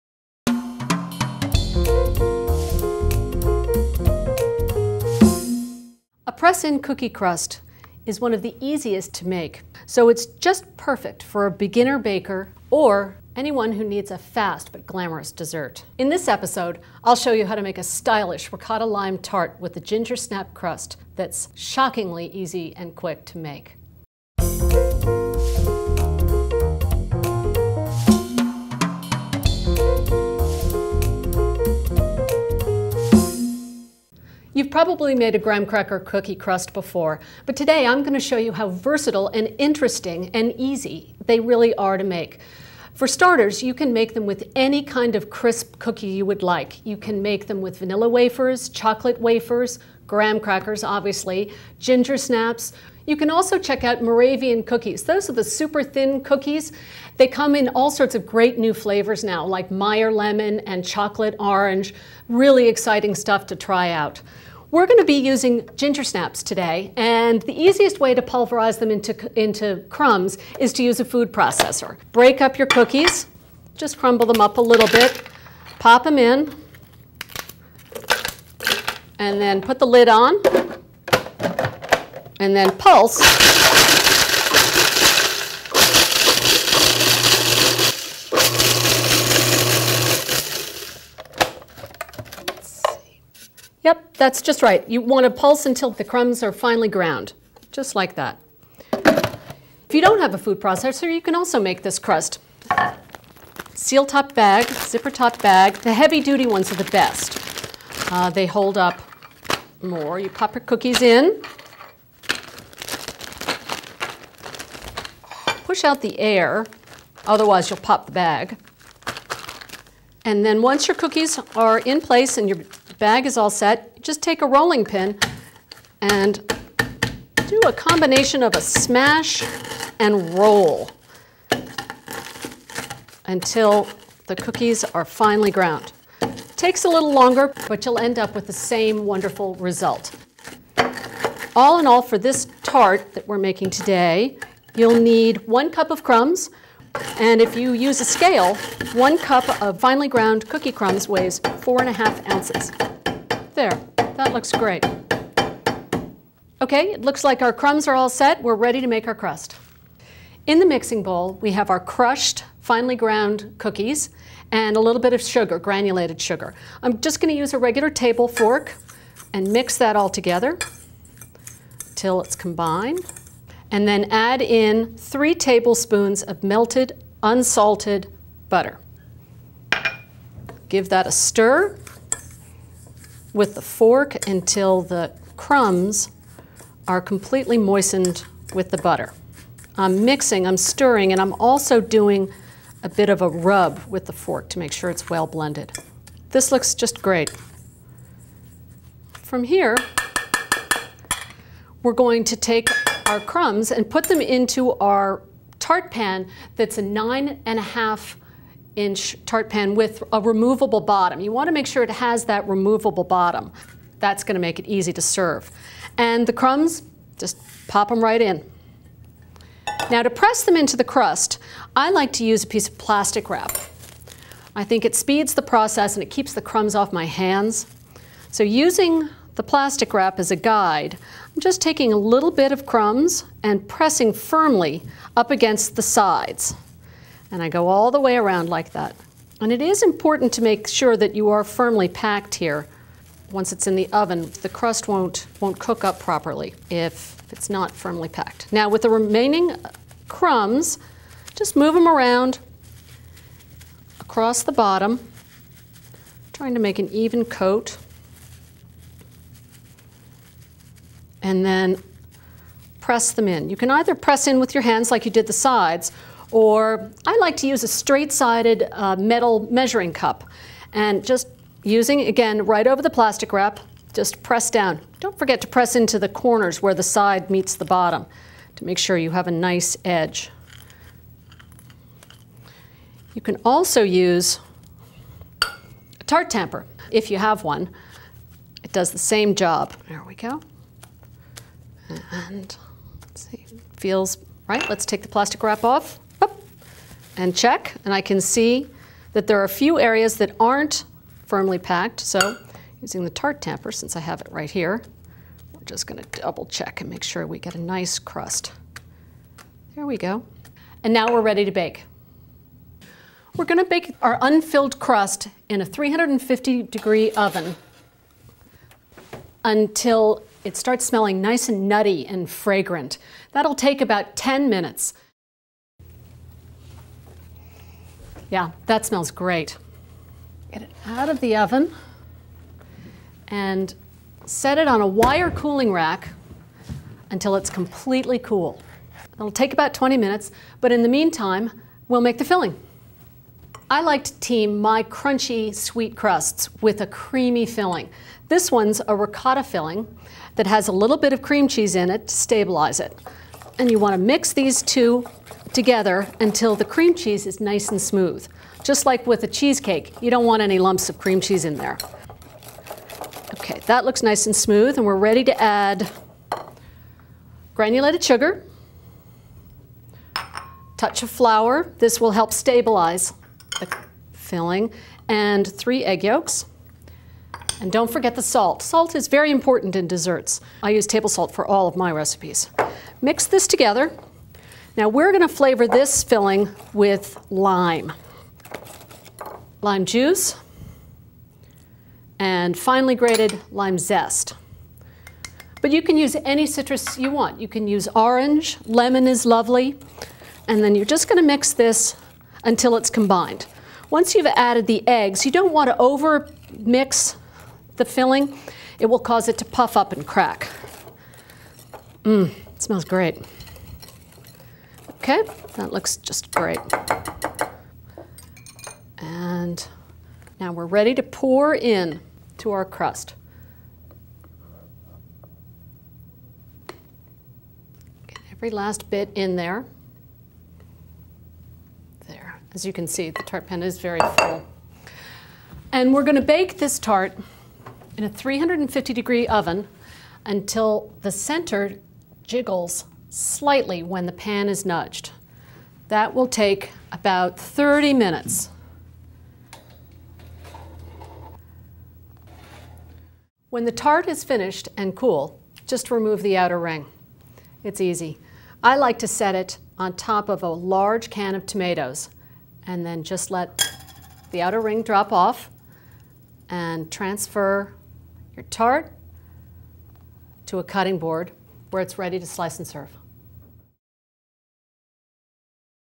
Cookie-Crust Tarts
Learn how to make simple but stylish tarts with a press-in crust made from ground cookies. Abby Dodge demonstrates a lime-ricotta tart with a gingersnap crust.